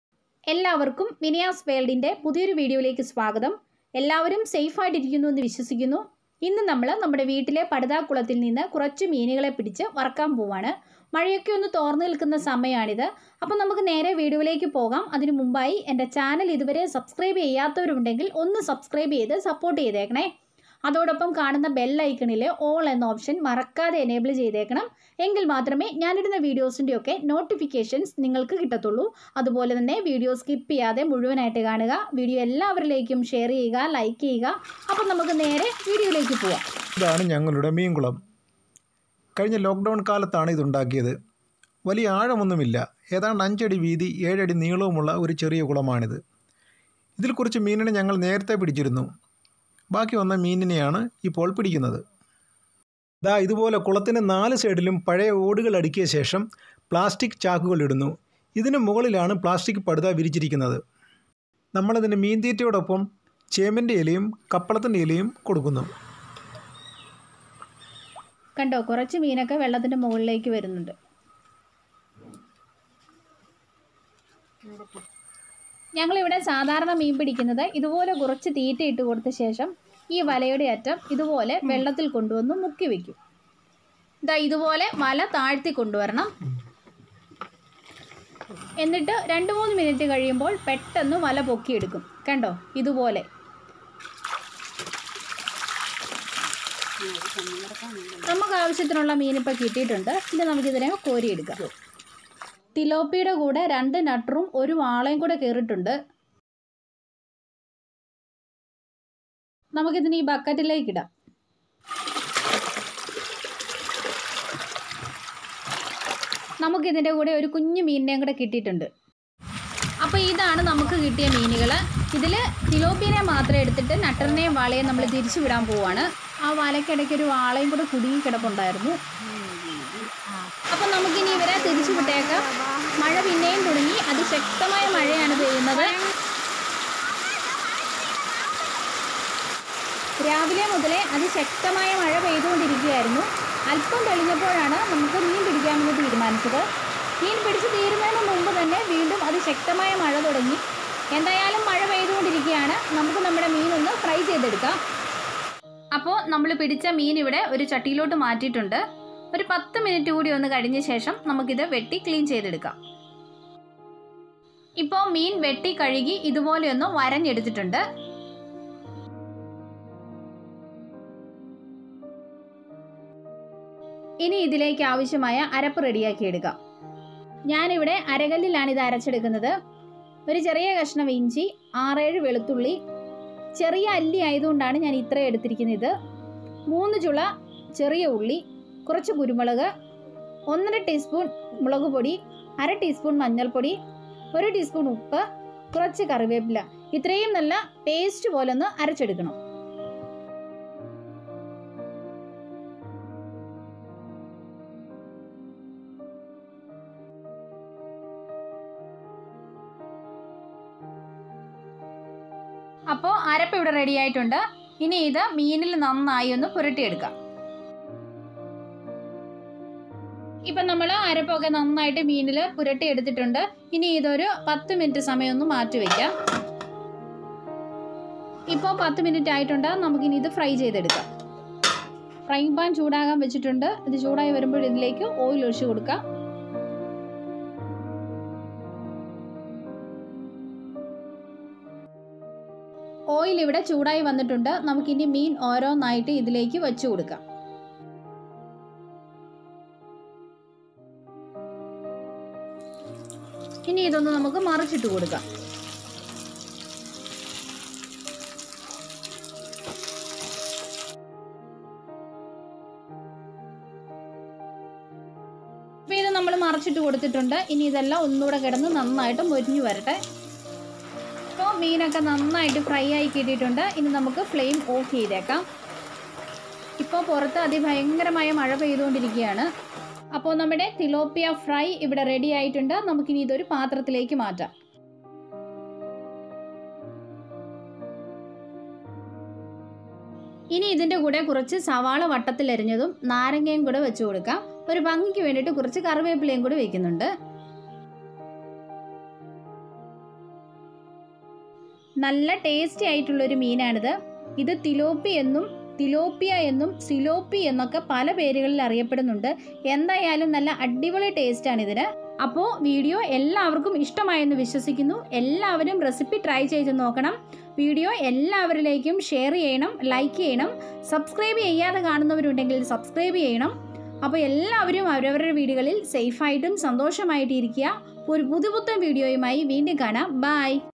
പെരുമഴയത്തൊരു മീൻ പിടുത്തവും മീൻ വറുക്കലും Vinaya's World/Malayalam/fish fry/fishing
ingredients

fish                            1kg
ginger.                       1 small piece
garlic.                         6 or 7
pepper.                       1 tbs
taurmeric.                    1/2 tsp
salt.                            1tsp